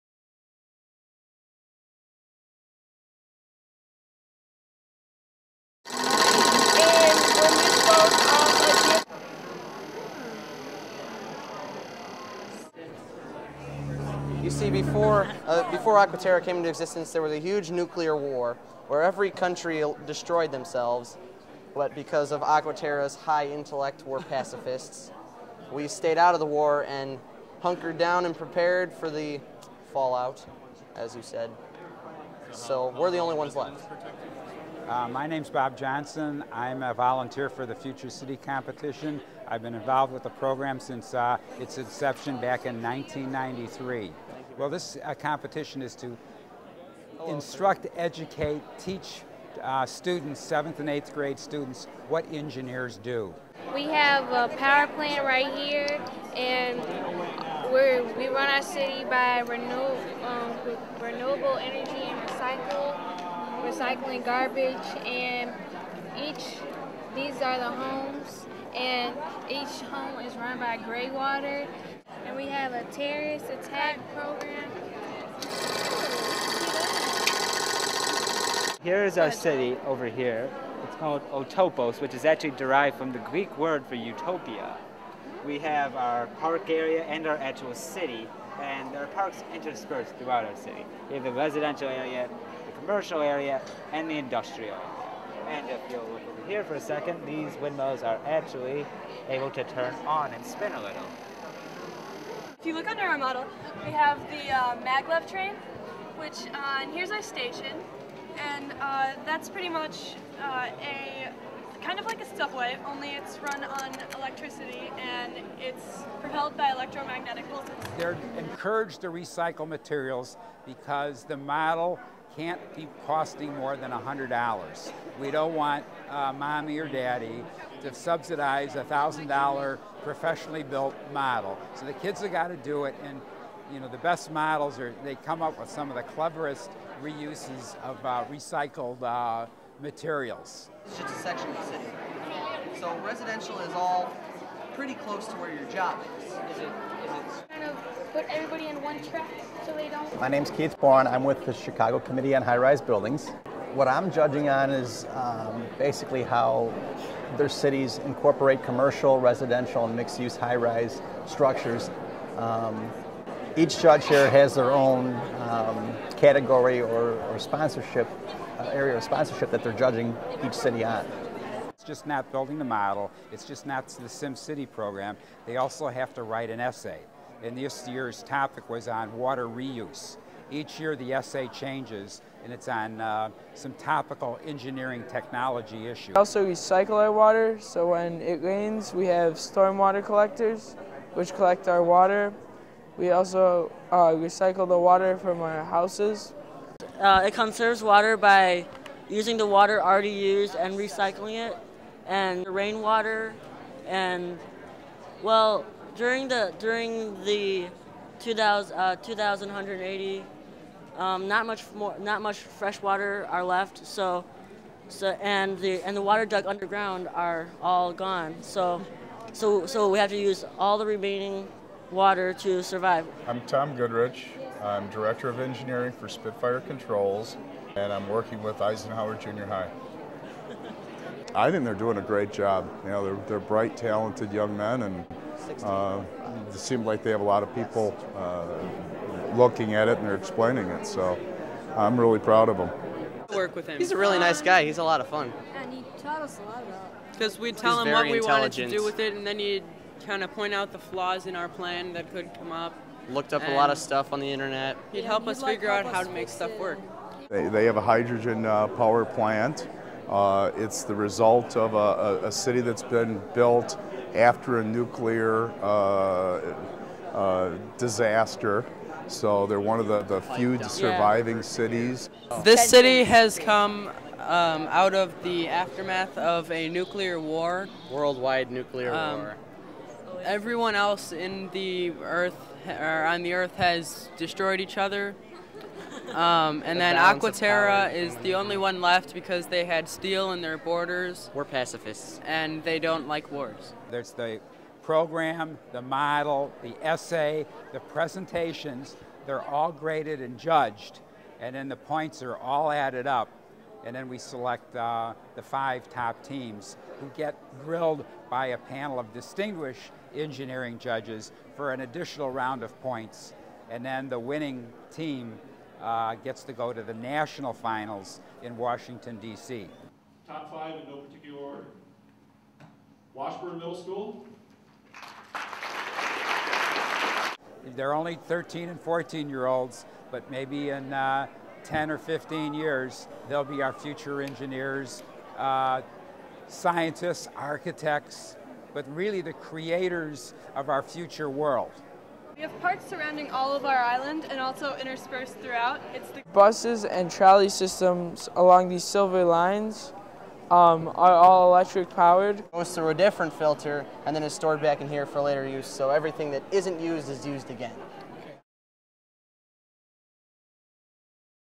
Future City Competition
Thousands of middle school students from across the country, including 120 students from Illinois vie for national recognition in the annual Future City Competition.

Videos, Interviews, and production by Cristina Aguirre.